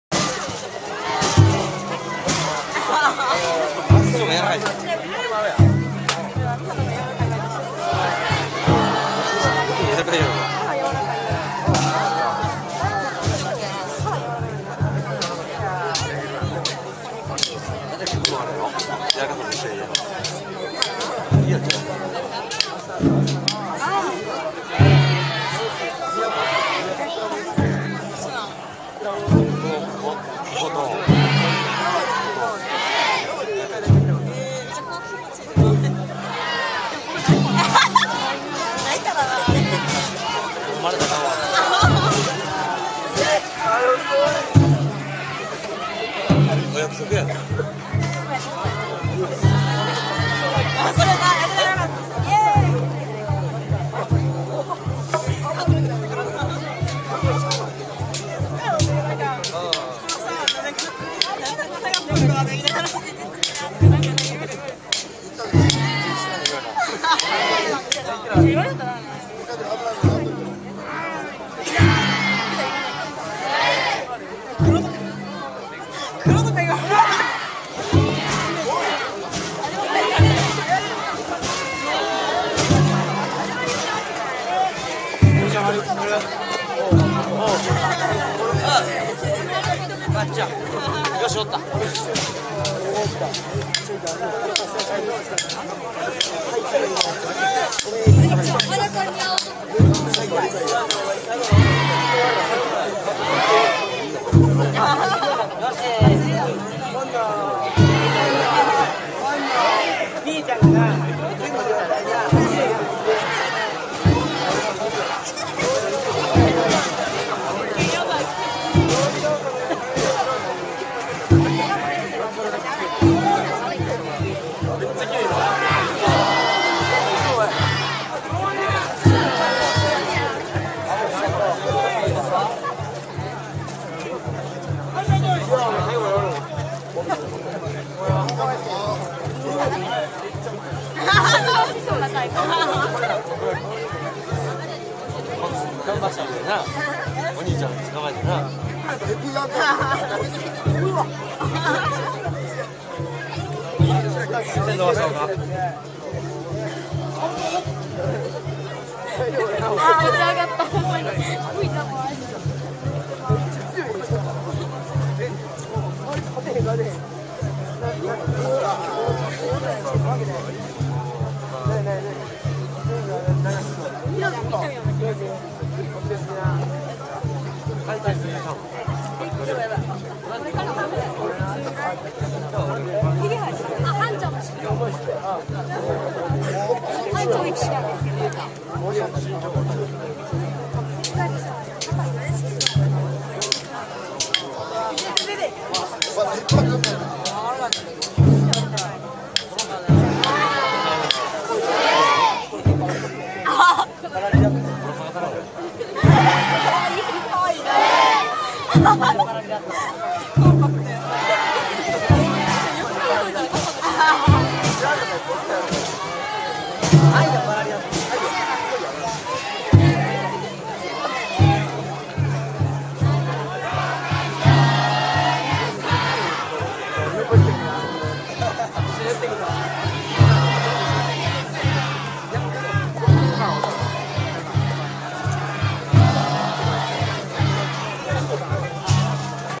'07 Nada no Kenka Matsuri Festival 12 / 灘のけんか祭り〜松原の屋台下山＠御旅山
"Nada no Kenka Matsuri Festival(Nada Festival or Nada Fighting Festival)" on October 14, 2007.
I took a picture of Matsubara's Yatai who went down Otabiyama.

See also:
The big push (CNN Traveller)

2007年10月15日の灘のけんか祭り。
これは、松原の屋台が御旅山山頂のお旅所から下山している様子を撮影したものです。地元の人らしき人が、こんなに潰れるのは珍しい、というくらい、担いでは潰れ、の繰り返しで練り場まで1時間くらいかかりました。